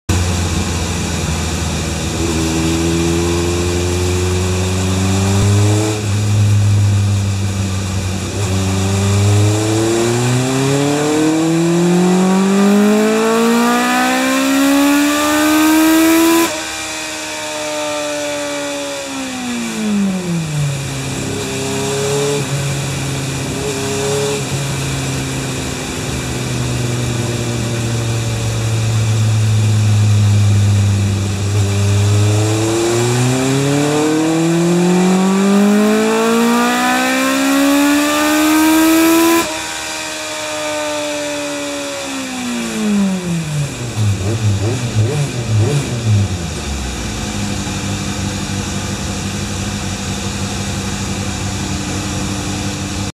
GPz750FのCR29にMノズル＆ＰＥＥＫ製リンクロッドを装着セッティング
７２ｐｓから７７ｐｓまで向上しました。